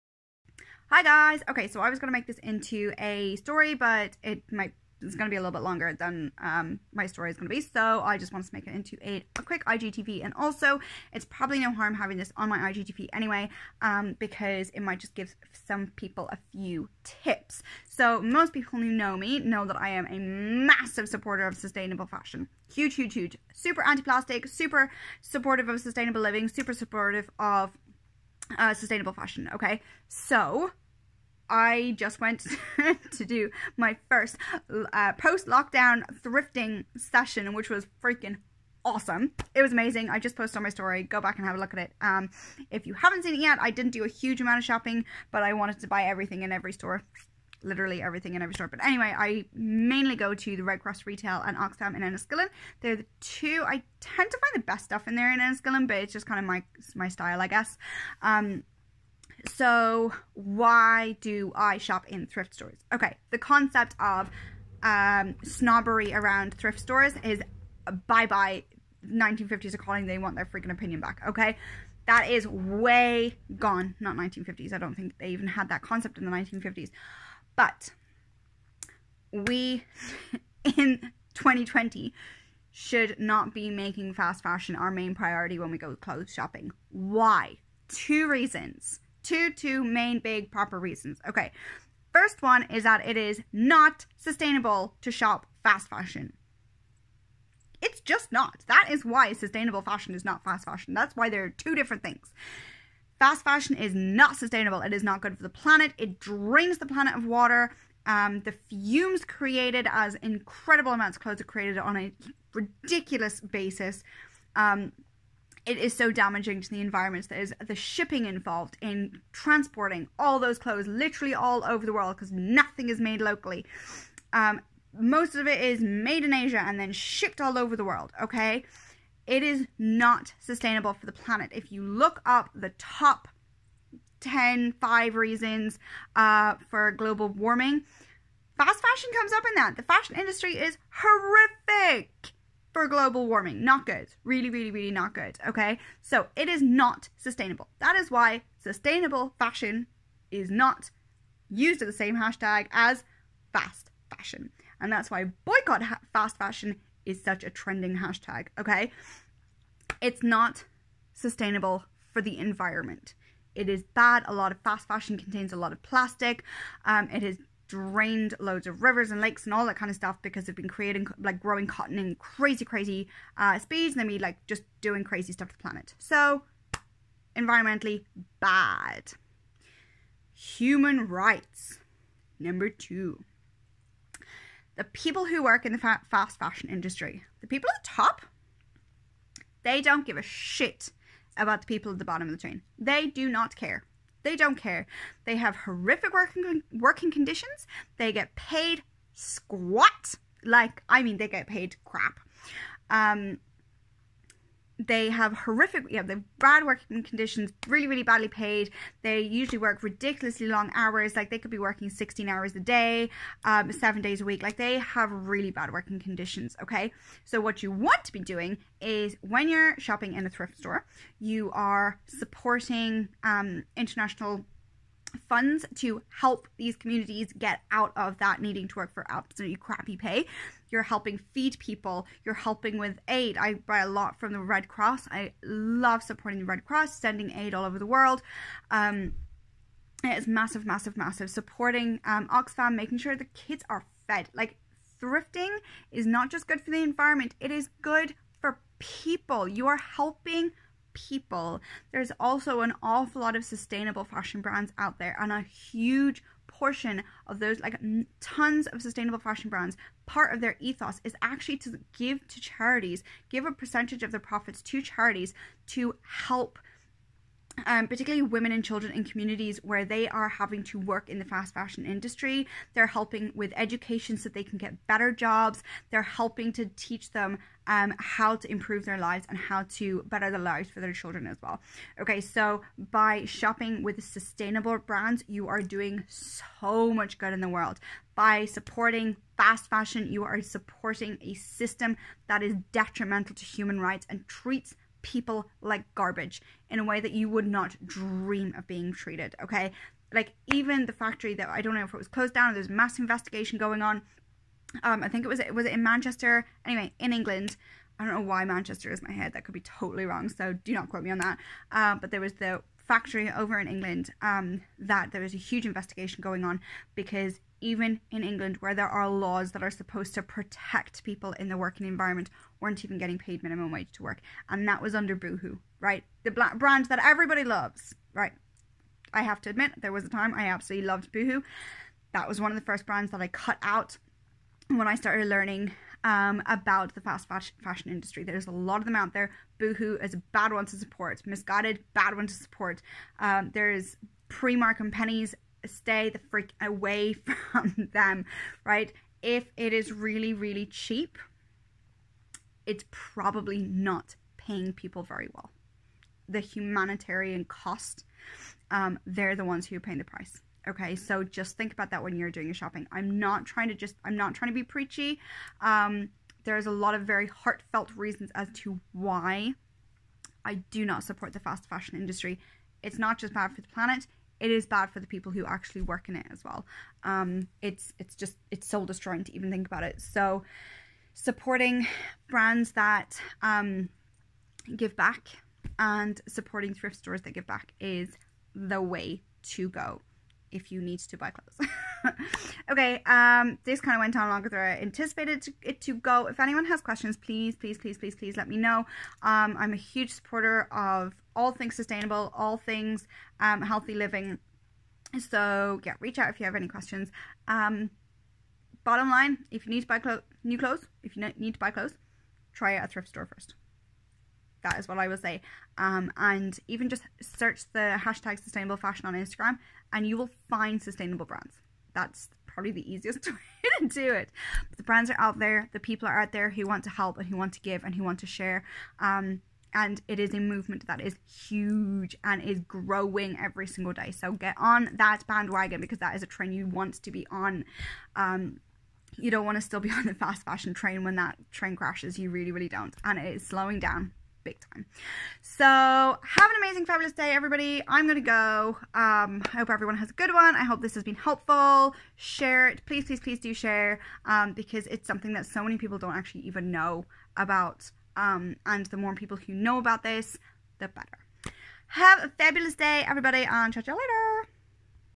Slow Down Your Fashion
Short chat about why I stopped supporting the fast fashion industry and how i support the slow fashion industry ❤️❤️

The fashion industry is one of the main contributors to our environmental crisis... If you could be a small part of that change without compromising your fashion, wouldn't you? 😍🌍